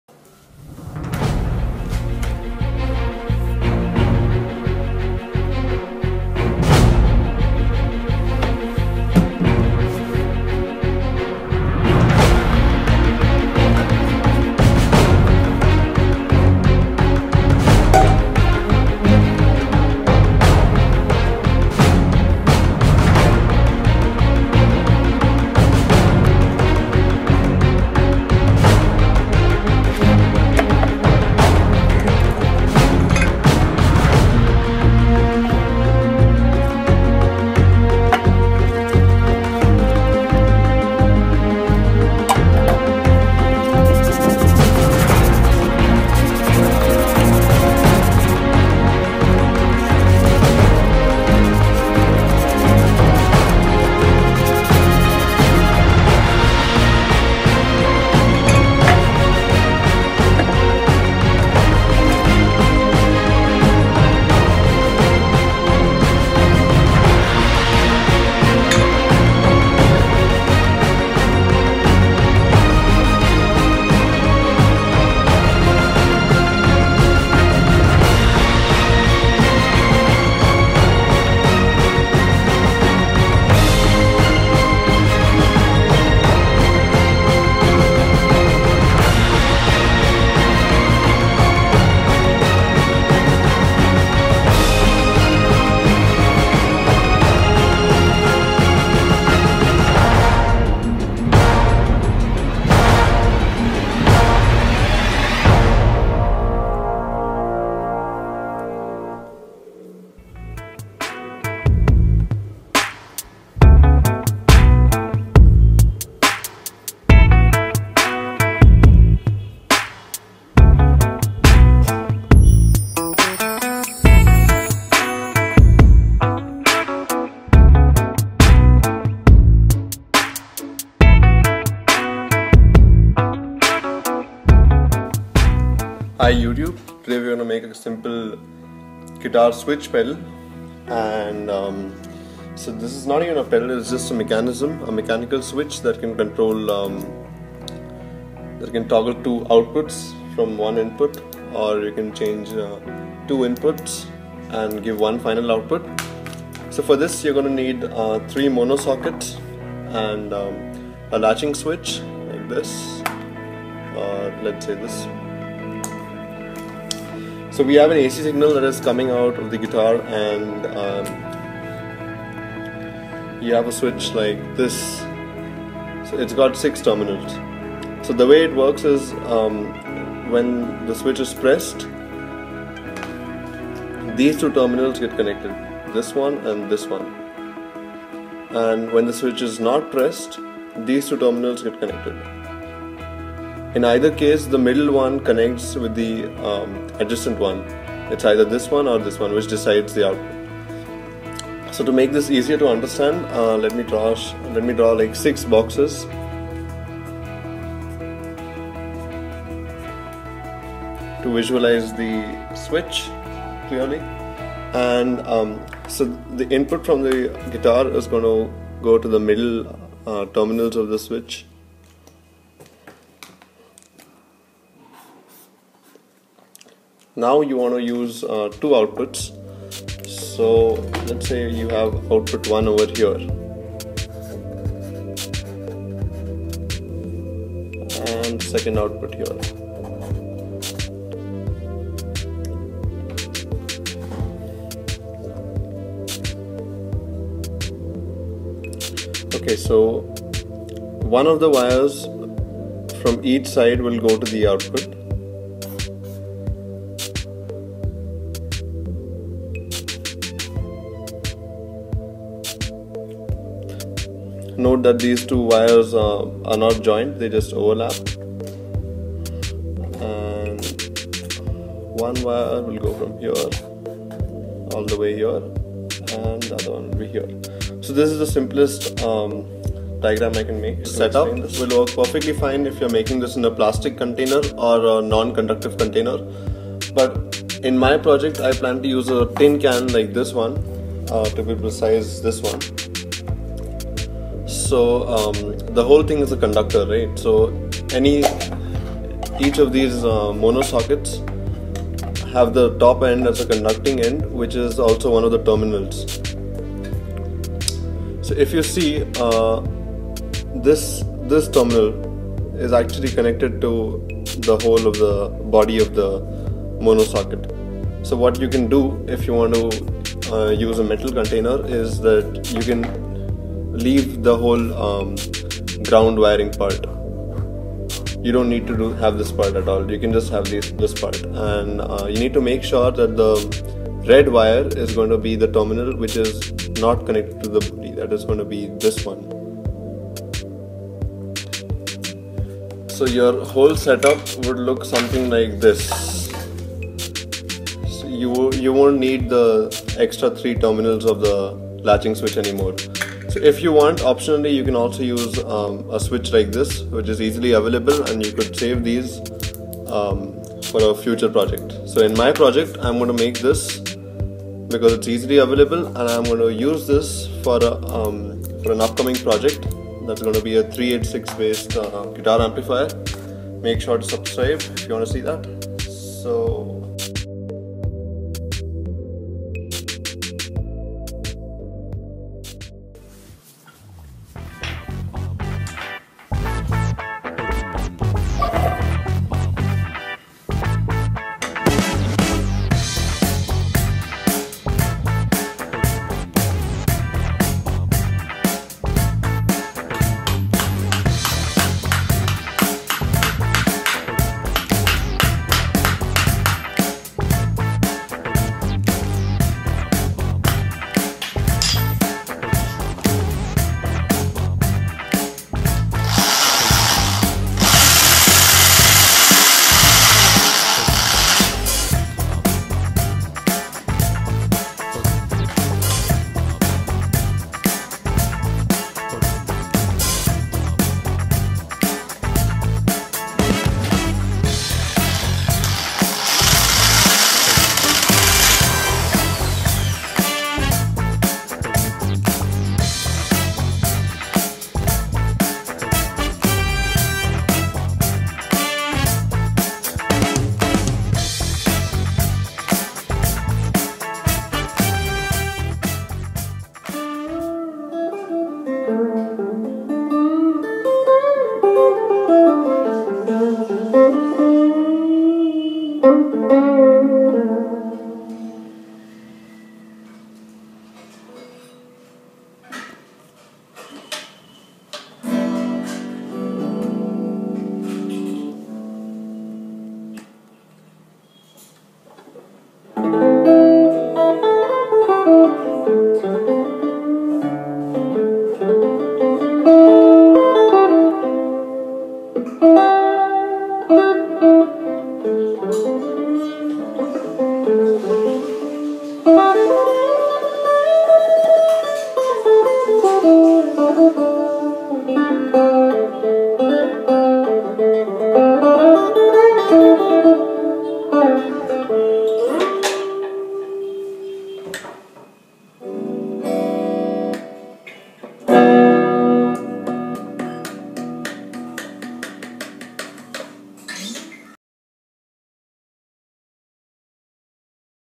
The Easiest DIY guitar pedal (A/B Switch)
Well, I had two guitars and one amp, and I would improvise on a backing track with a friend in turns, the switching became much more efficient! Yes, using a latching guitar foot-switch would have made it the ideal pedal but to keep the whole system inexpensive I've used a general toggle switch,
The A/B switcher can be used in 3 ways,
1.For switching between 2 different amplifiers,
2.For switching between 2 guitars connected to the same amplifier,
3.For switching between two different pedal setups.
And probably many more uses that I haven't thought of
I plan to make a simple guitar amp based on the lm386 IC, and keep the circuitry simple with as many possible features,
Stay Creative

Cheers
Siddharth